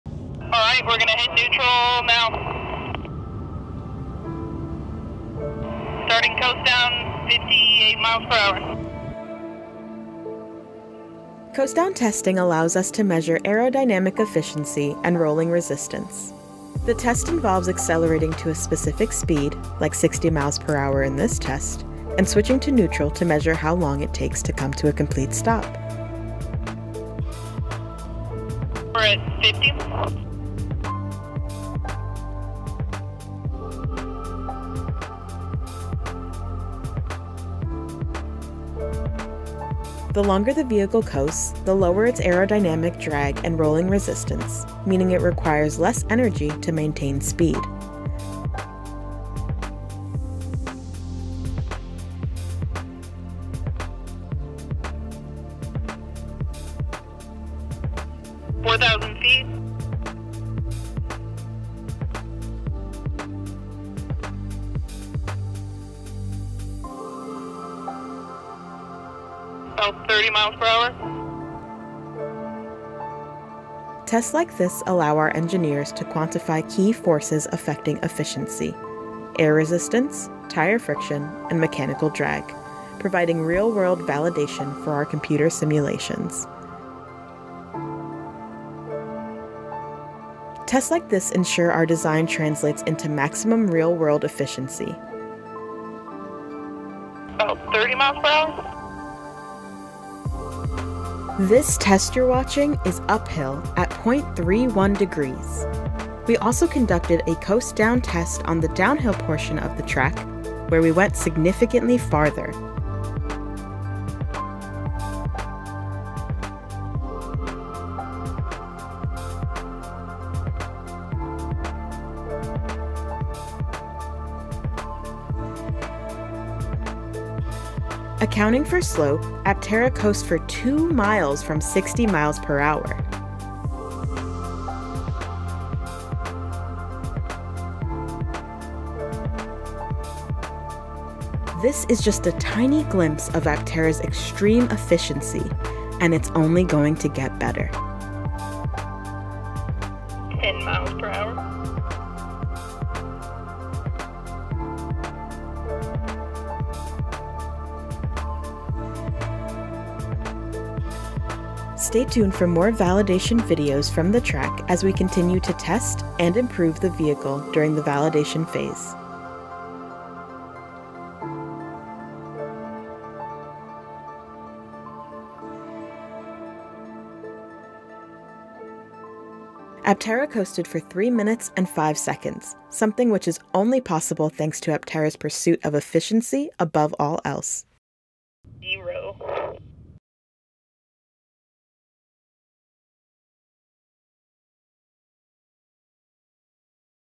Aptera — How Long Will It Coast?
We took our first production-intent validation vehicle to the proving grounds for coastdown testing—and the results left us in awe. This is just a tiny glimpse of Aptera's extreme efficiency—and it's only going to get better.